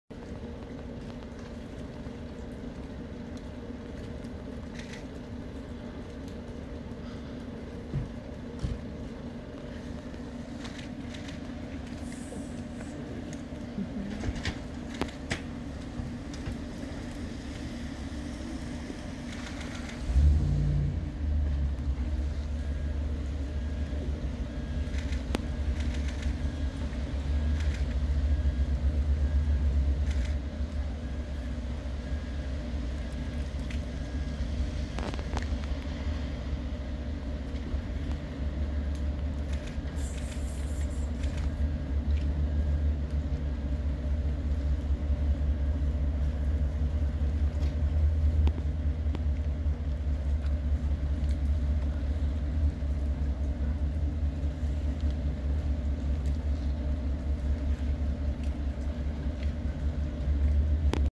Beautiful Fox and her 3 kittens
We visited Grand Teton Wyoming. These foxes were living underneath the laundromat at Culter Bay.